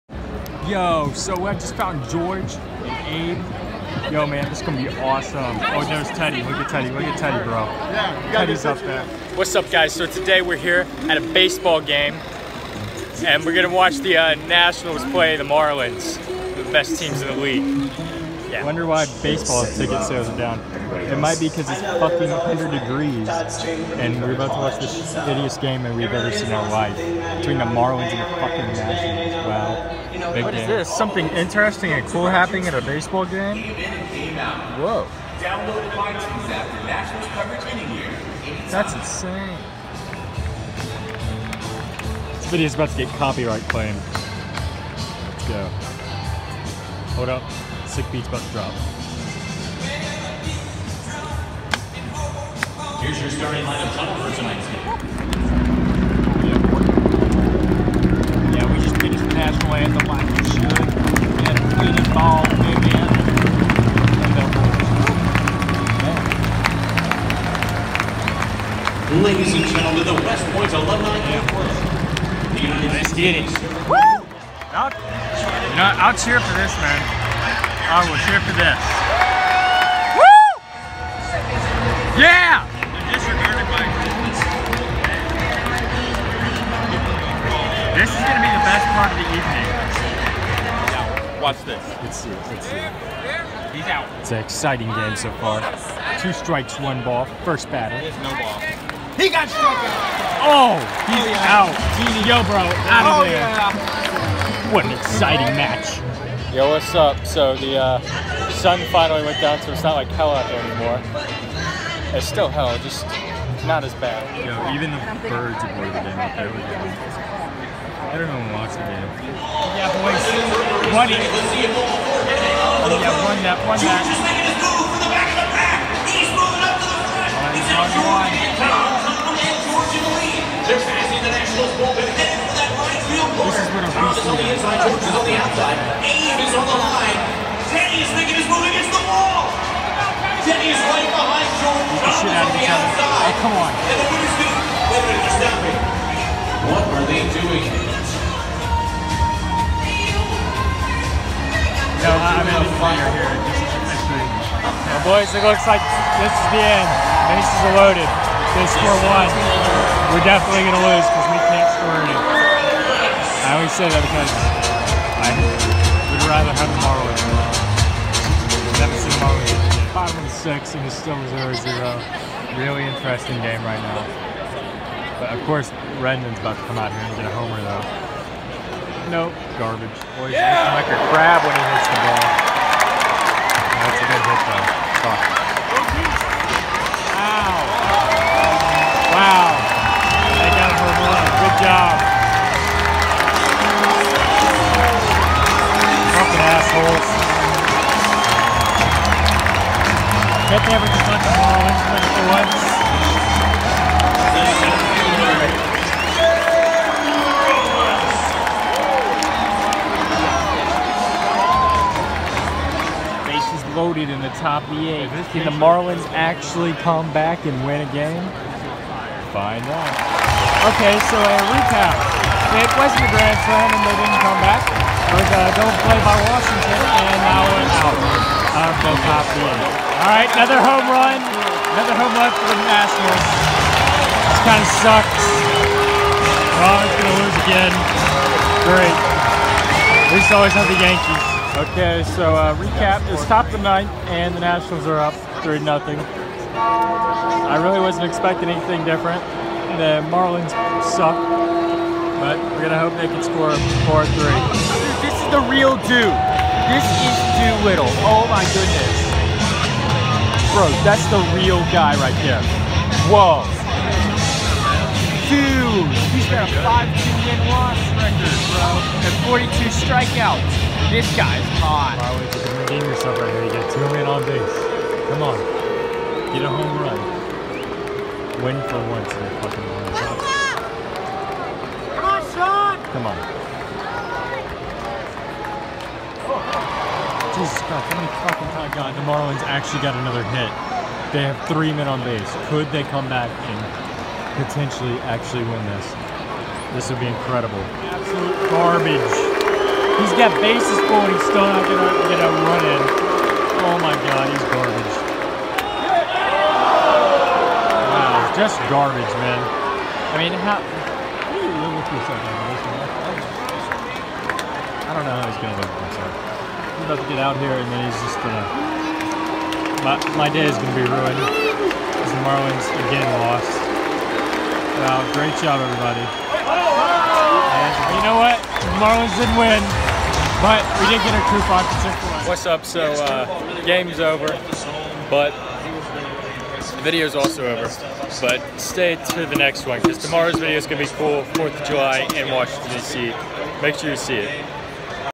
The Most Entertaining Game in the World
I go to a Washinngton Nationals and Florida Marlins game. The result is about as exciting as you would think.